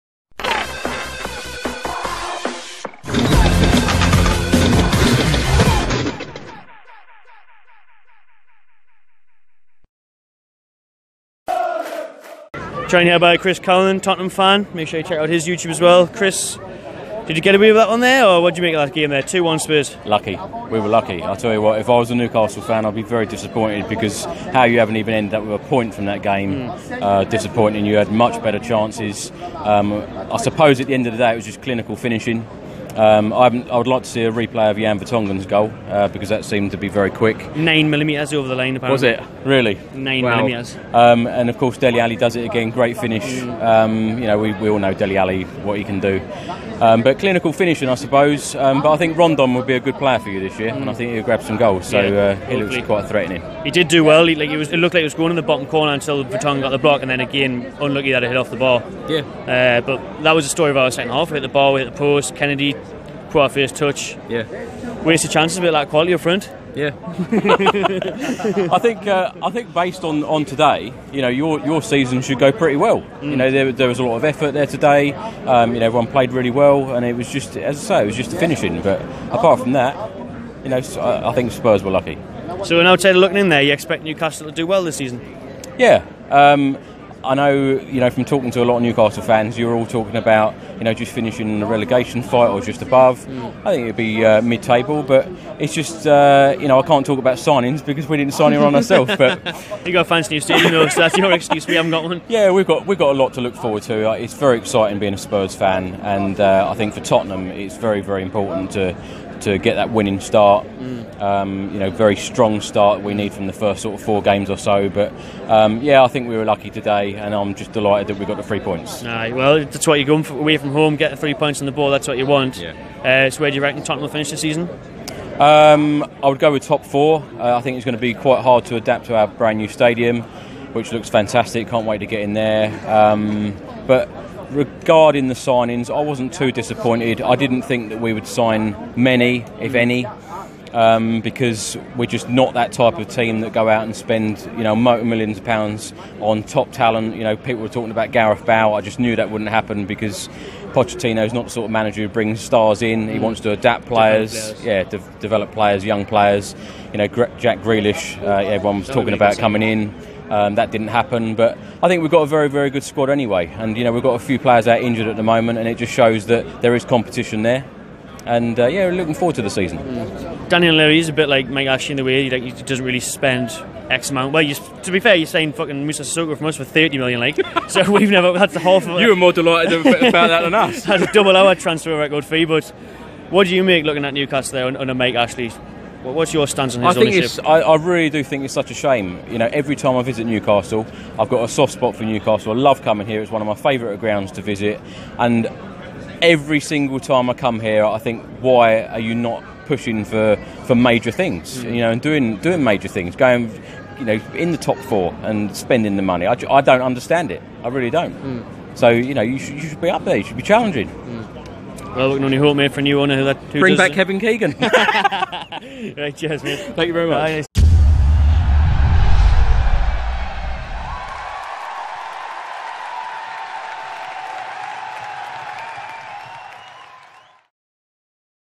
TALKING TO "THE MAPGIE CHANNEL": "I'm Delighted We Got the 3 Points" - 11 August 2018
+ CONTENT:
* Tottenham fan Chris Cowlin chats with "The Magpie Channel" about Tottenham's win at Newcastle United.